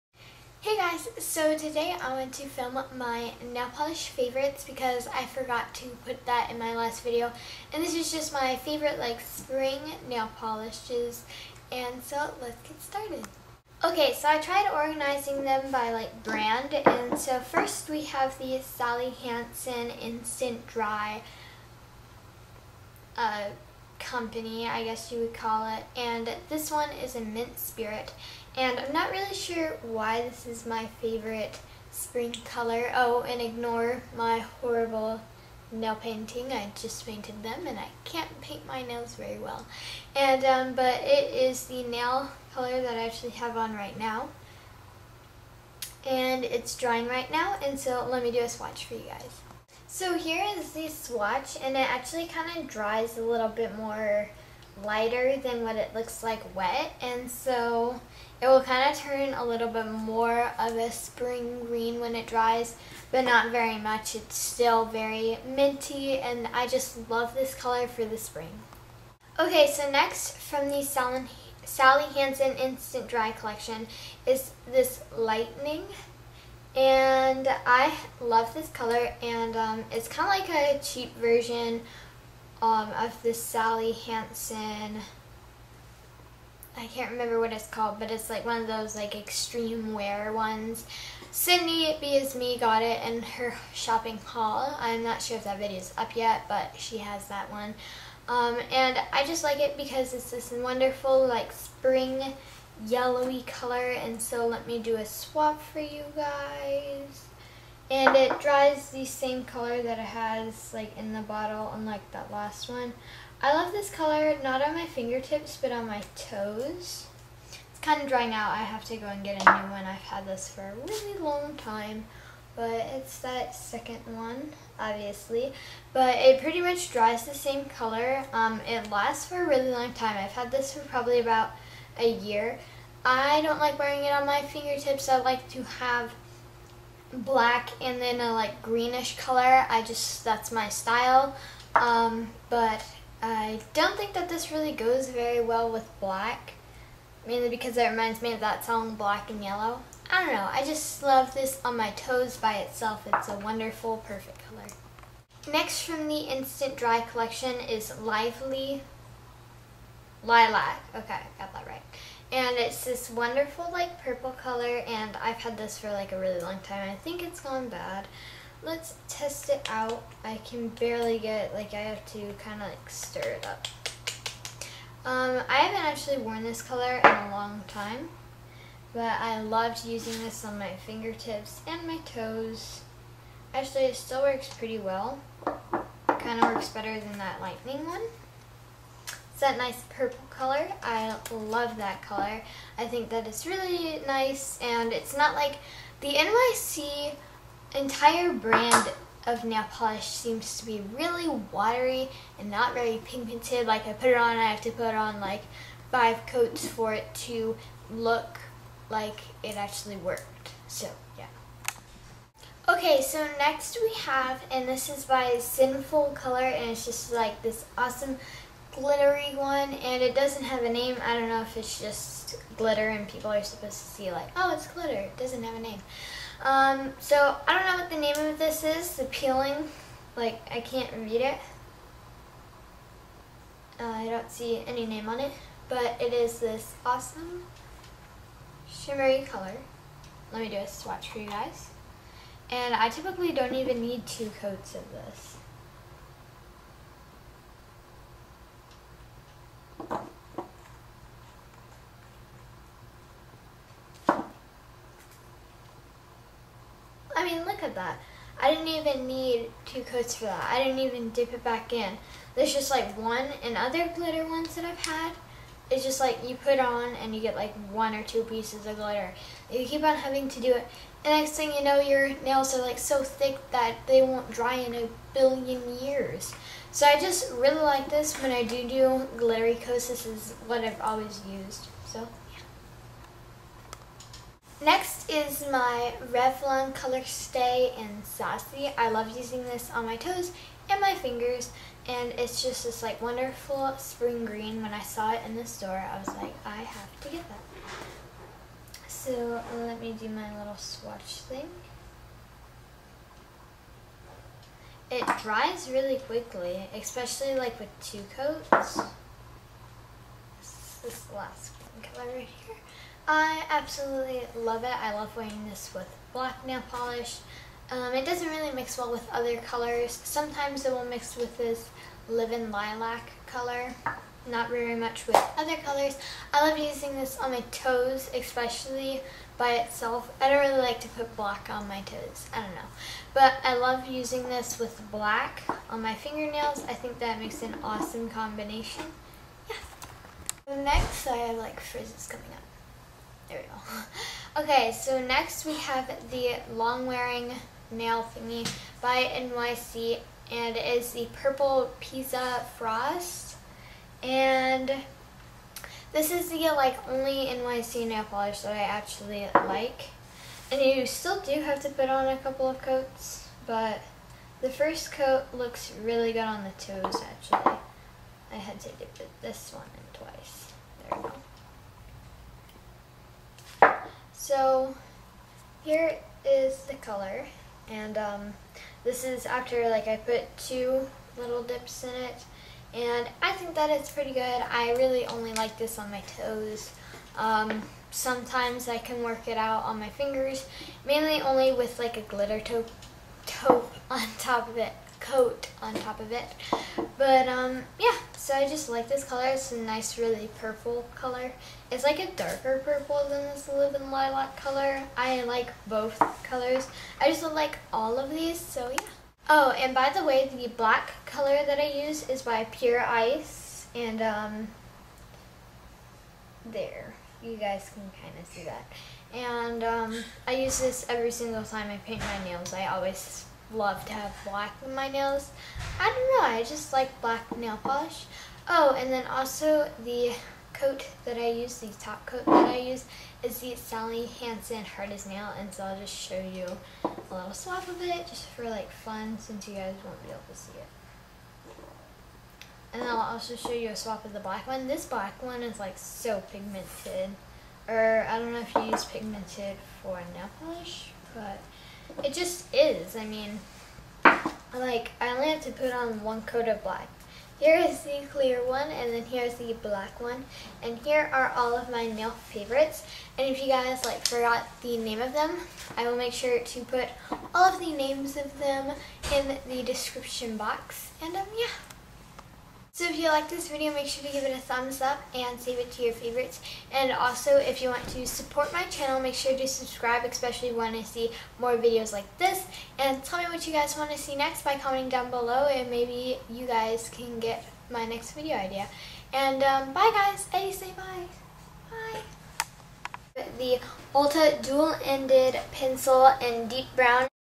May Nail Favorits!
tell me: What do you want me to post next?

My May Spring Nail Favorites:
N.Y.C: Purple Pizzazz Frost
Revlon: sassy
SallyHarsen Insta-DriL: Lightening, Lively Lilac, Mint Spint, Crystal Clear.
Sinfulcolor: Some glitter Coat
Pure Ice: Some Blake Coat

See you guys later! love you guys!! :)

BYEEEE!